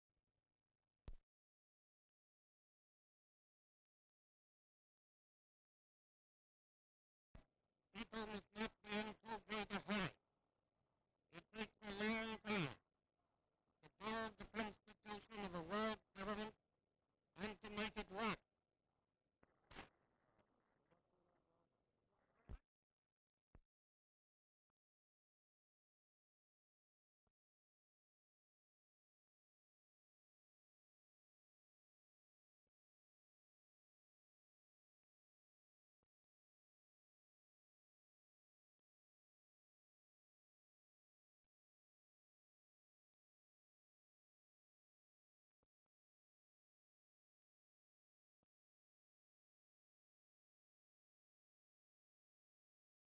Bevin In New York (1946)
Foreign Secretary Ernest Bevin travels to New York for the Big Four meeting.

M/S as Mr and Mrs Ernest Bevin walk down the gangplank of the liner "Aquitania". C/U of Foreign Secretary Bevin speaking into microphone saying it takes time to build constitution of world government.  M/S as he walks towards the camera in the Waldorf Astoria Hotel for the Big Four meeting, Bevin takes his seat at the conference. Russian Minister Vyacheslav Molotov enters, C/U's of him. Various shots of De Murville, Molotov, Andrei Vyshinsky, and James Byrnes.

Possibly connected with 46/90 - MD. Very poor sound - Neg sound - MD
 FILM ID:1487.43